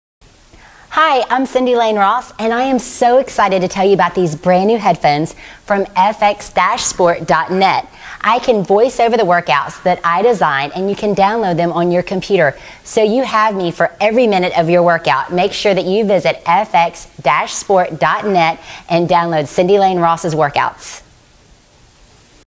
Introduction Video for FX-Sport.net
Introducing these new headphones with my voiced over workouts to help you reach your fitness goals.